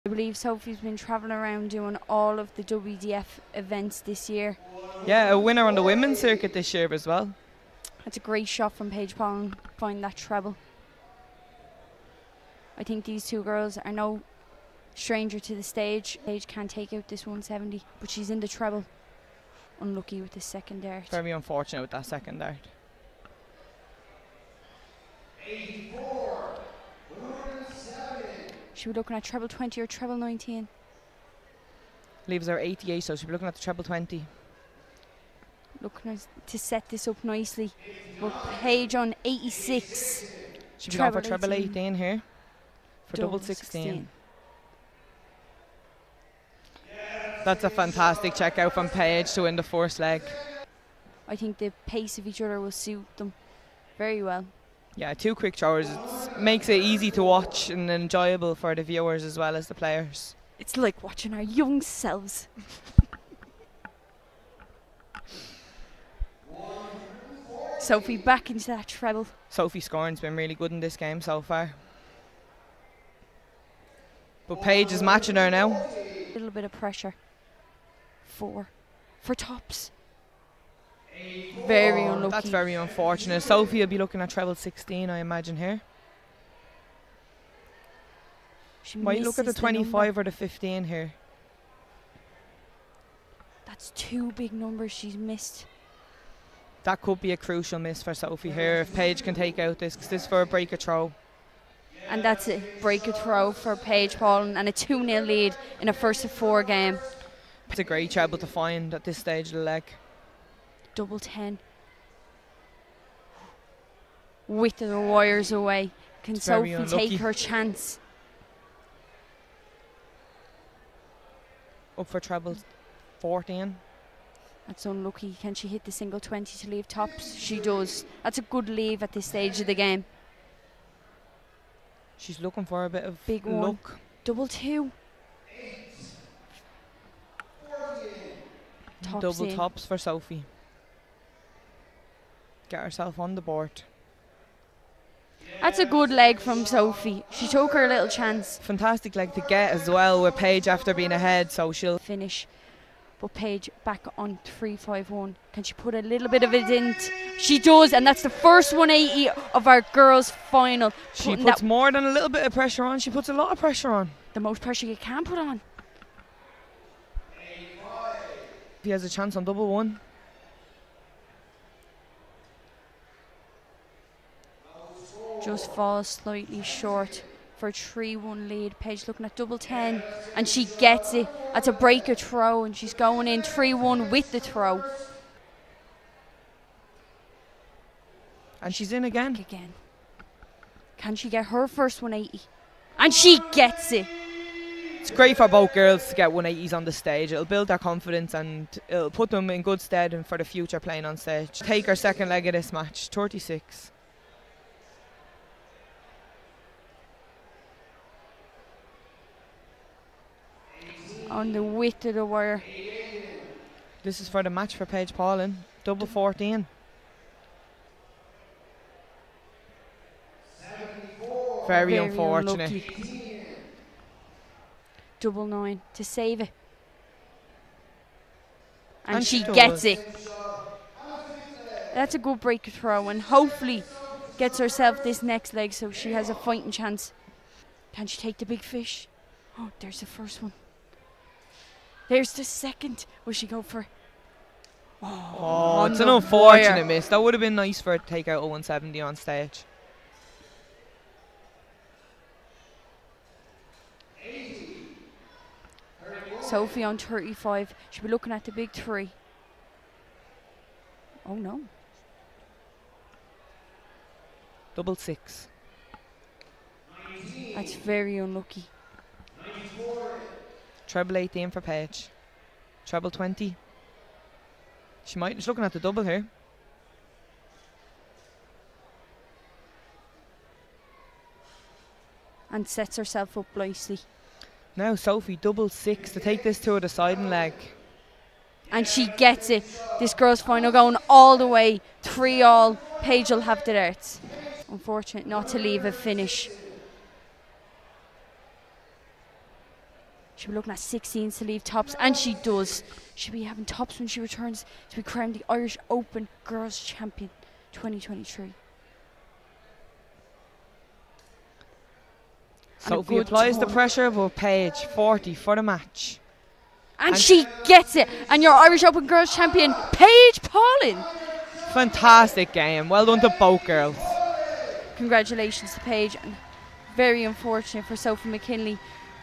Pauling Superb as she adds another title!
Paige Pauling shocked no one when she claimed another big title at the INEC in Killarney

The current reigning world cup champion was pushed all way by Sophie McKinlay